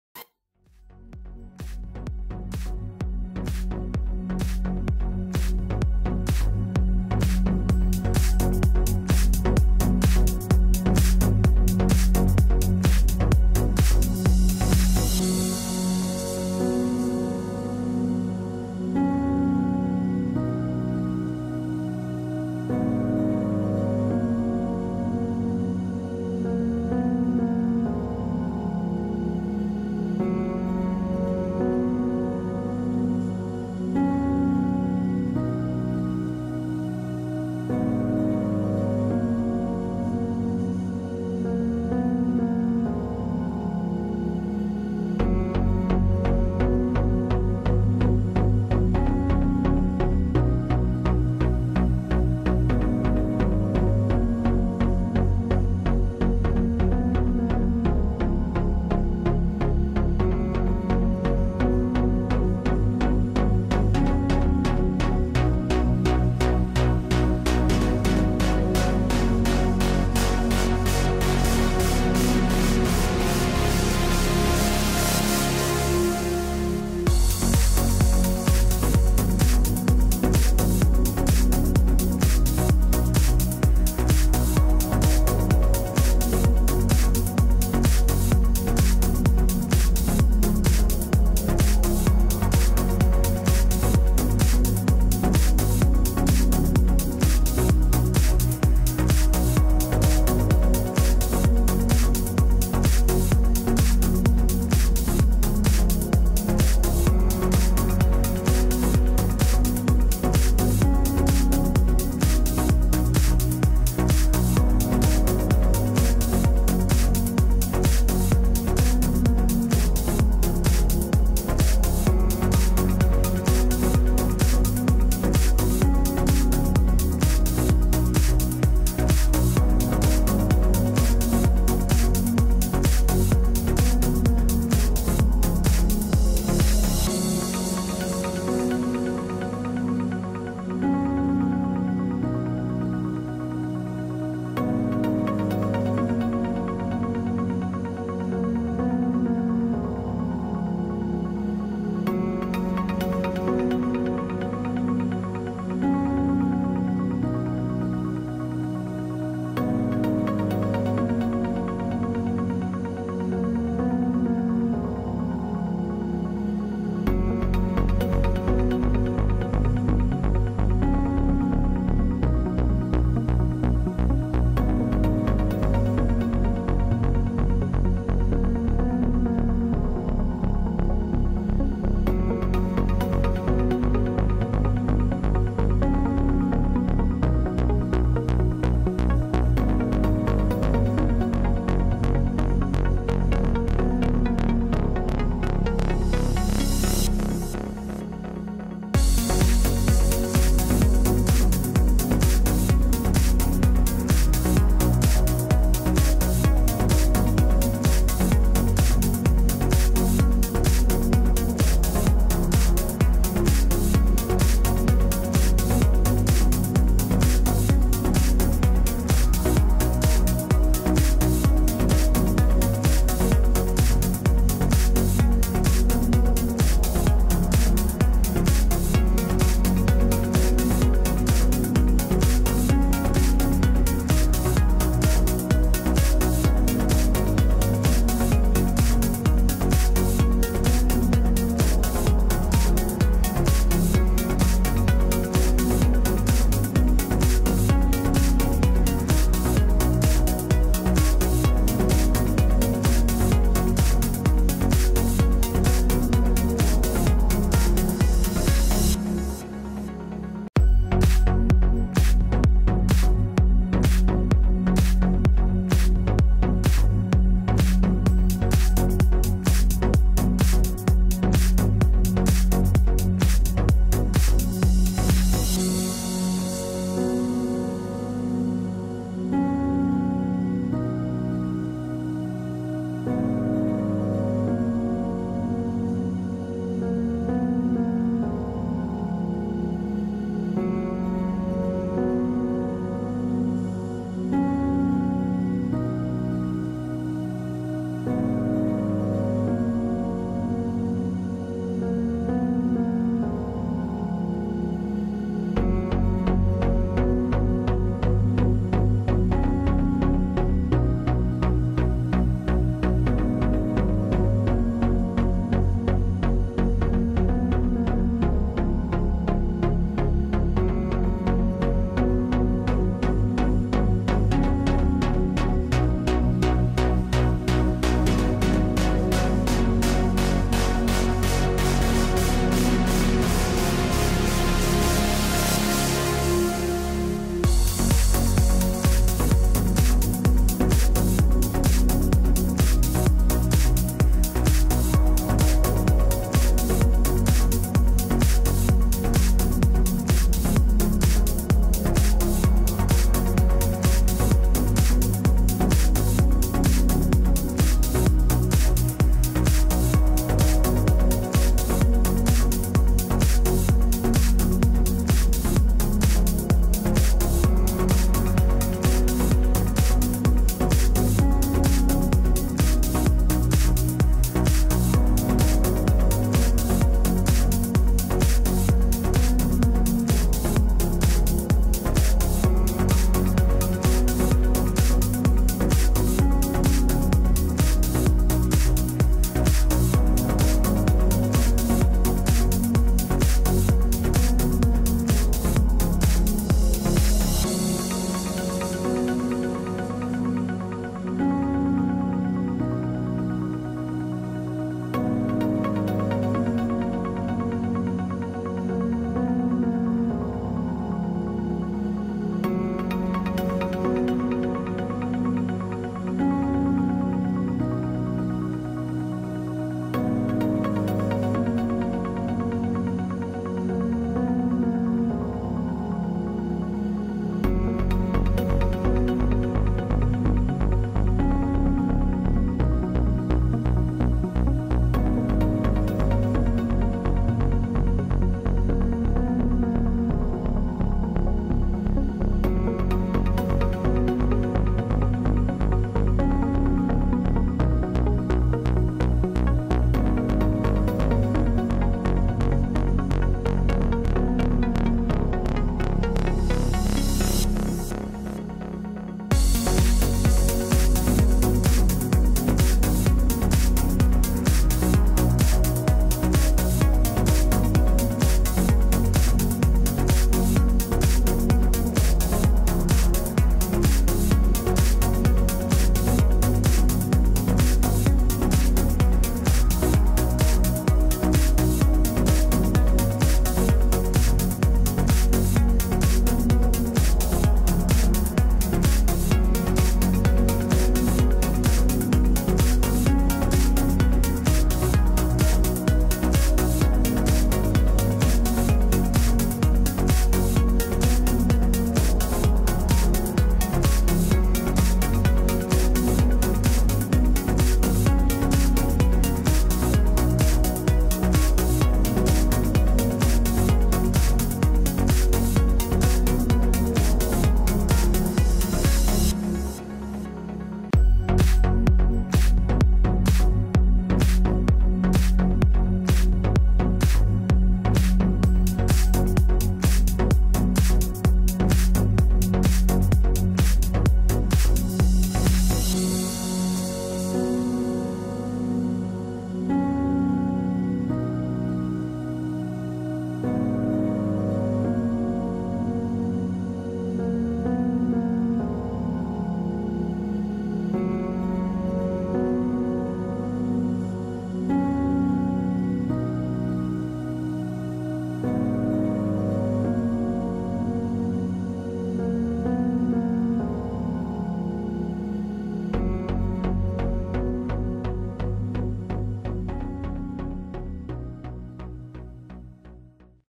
Intuitive Drawing #41 / Piranha Fish / Soul Expressionism / Abstract Doodle / Daily Art Therapy
Soul Expressionism drawing, titled "Piranha Fish" created with a Sharpie on 110lb cardstock.  (daily art therapy ) Satisfying art form from the soul.  From our series "Intuitive Drawing" #41
The Art of Spiral Drawing

You can follow me on:  (links below)